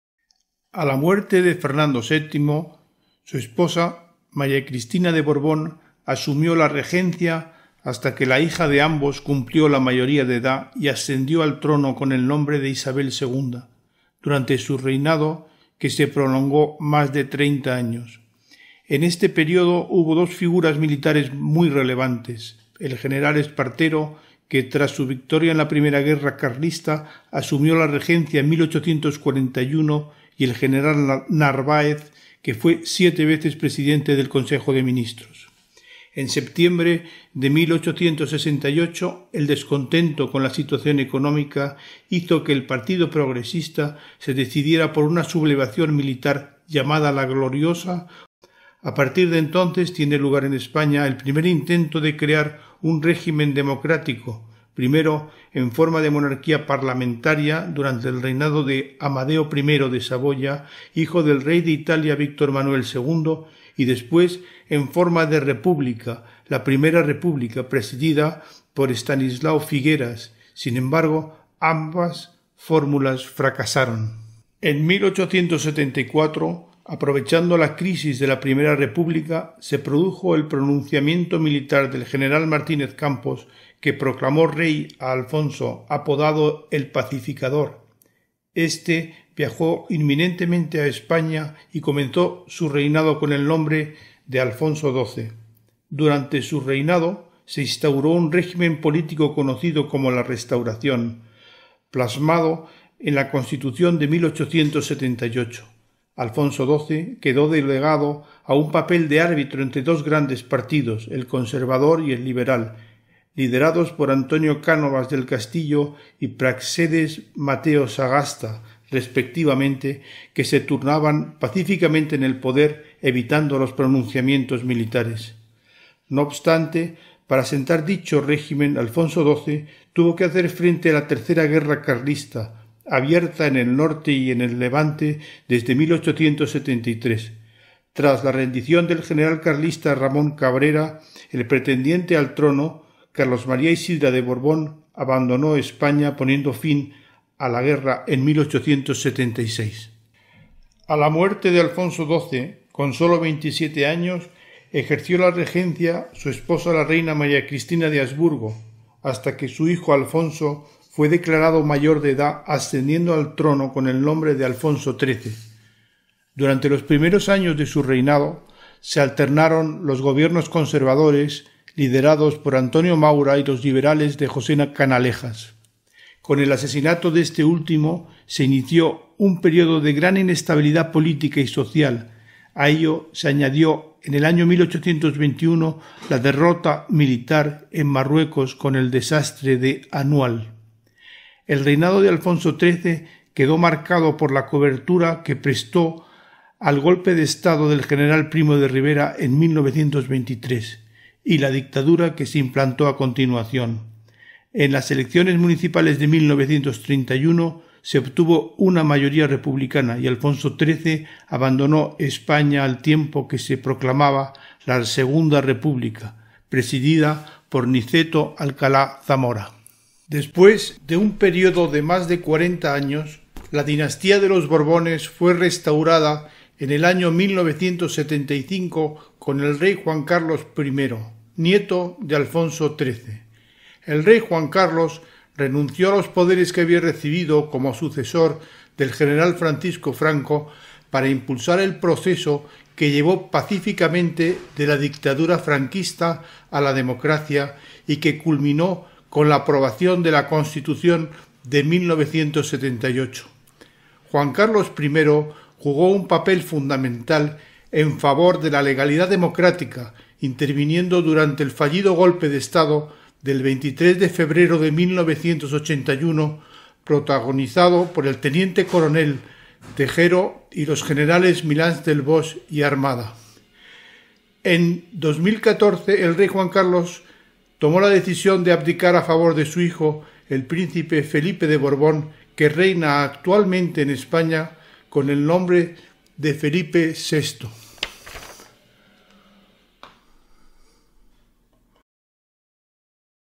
Dinastía de los Borbones: Parte 2 🇪🇸
La Dinastía de los Borbones: un recorrido por la historia Monárquica de España.

Desde el tumultuoso reinado de Isabel II hasta la actualidad con Felipe VI, la historia de España ha estado marcada por la presencia de la Dinastía de los Borbones. Isabel II ascendió al trono en 1833 en medio de una España convulsa, con constantes luchas internas y figuras militares como el general Espartero y Narváez. Sin embargo, su reinado enfrentó desafíos políticos y económicos que culminaron en su exilio durante la Revolución de Septiembre en 1868. Este periodo también vio el intento fallido de establecer una monarquía parlamentaria y la breve Primera República.

Alfonso XII, conocido como "el pacificador", tomó las riendas del país en un momento crítico tras la crisis de la Primera República. Durante su reinado se instauró el régimen de la Restauración, donde se alternaban pacíficamente en el poder dos grandes partidos políticos, el conservador y el liberal. A pesar de las tensiones políticas, Alfonso XII logró hacer frente a la tercera guerra Carlista y sentar las bases para un periodo de relativa estabilidad.

Con la llegada de Alfonso XIII, España entró en una etapa de inestabilidad política y social, agravada por el desastre del Annual en 1921 y la instauración de la dictadura de Primo de Rivera. El papel del rey en estos años fue controvertido, marcado por su apoyo al golpe de Estado y la posterior dictadura. Sin embargo, su reinado llegó a su fin con la proclamación de la Segunda República en 1931, que llevó al exilio a Alfonso XIII y a la caída de la monarquía en España.

📌 Momentos clave de la Dinastía de los Borbones:

0:13 Isabel II
01:17 Alfonso XII
02:36 Alfonso XIII